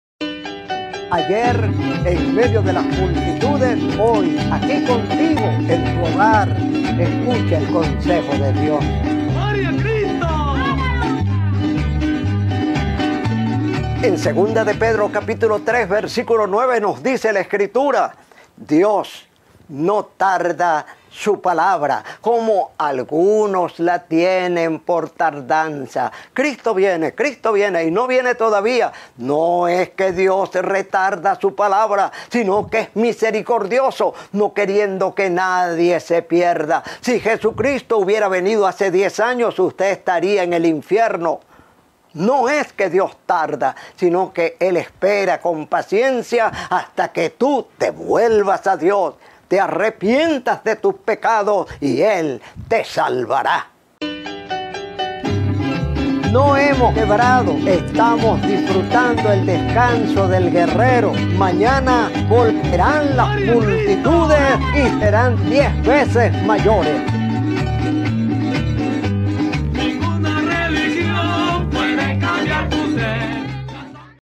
HNO PUERTAS #394 | DIOS NO TARDA | PRÉDICA BÍBLICA | MICRO HNO PUERTAS | LUZ TV
Dios no es lento y no actúa de acuerdo a nuestra medición del tiempo. Dios esta esperando que más pecadores se arrepienten y se vuelvan a él.

pastor Jaime Puertas:
Dios fiel a sus palabras
2Pedro 3.9